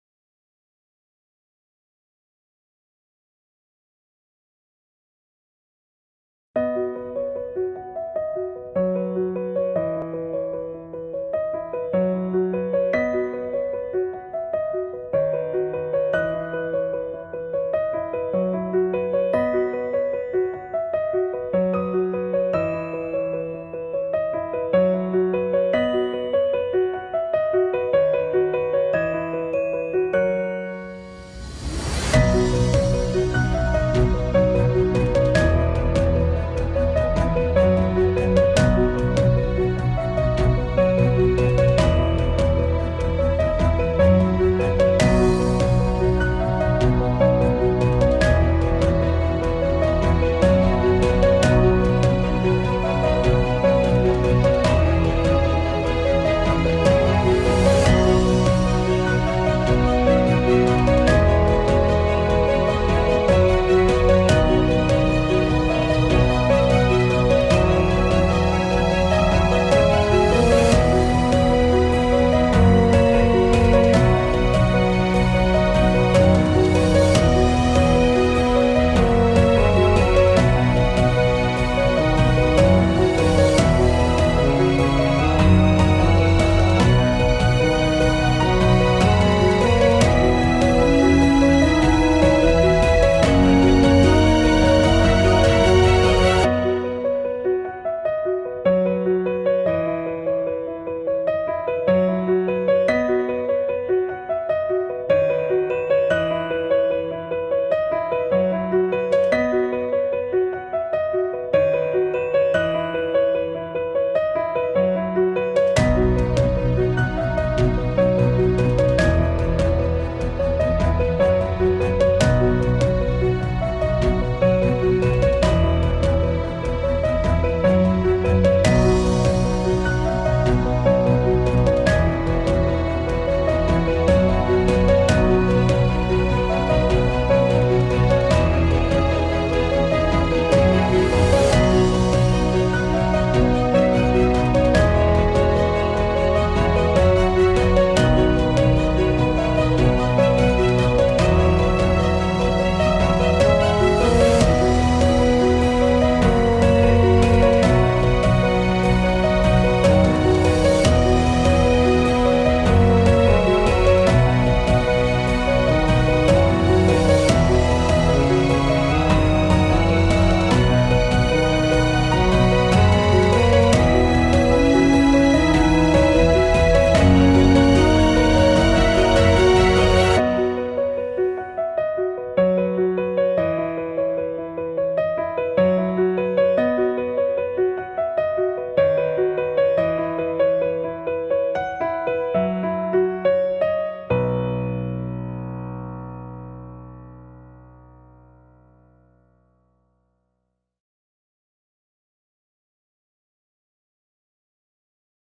Canon Grootformaat printers uit de PRO serie
Het assortiment professionele grafische printers van Canon, de PRO-serie